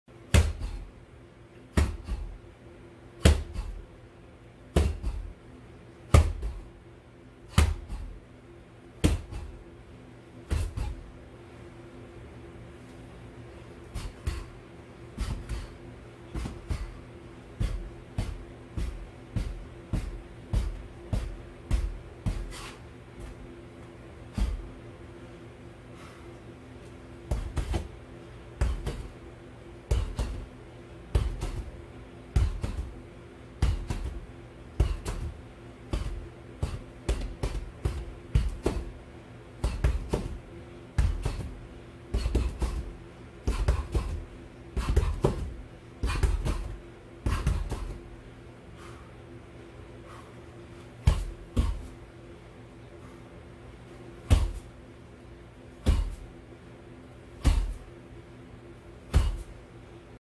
I Am No Professional, Zombieland Training; Beed-Fu boxing basics and low kick
week 23 Zombieland Training
title says it all
Jesus comes quickly
bless ya'll